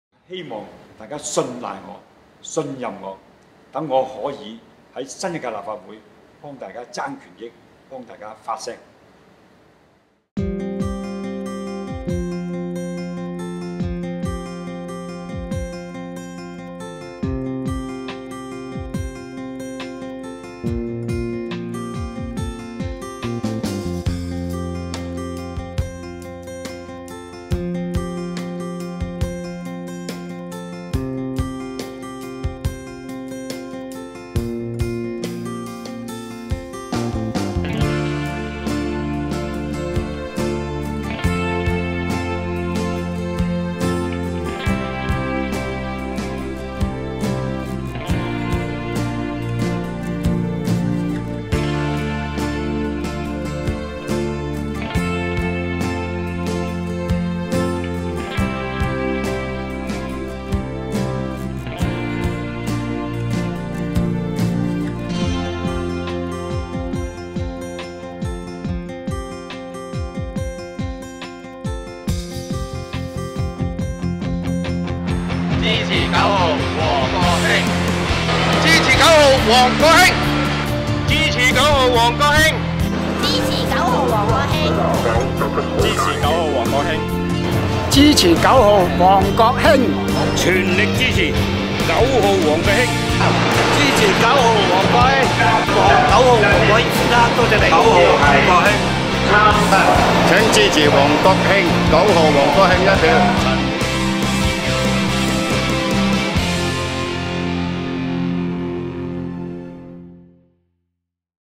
支持9號工聯會王國興
希望大家信賴我，信任我。
等我可以新一屆立法會幫大家爭權益，幫大家發聲。

2012立法會選舉香港島候選人
9號 工聯會王國興團隊選情告急!
懇請各位全家總動員，全投工聯會。9月9日投9號!
一票都好重要!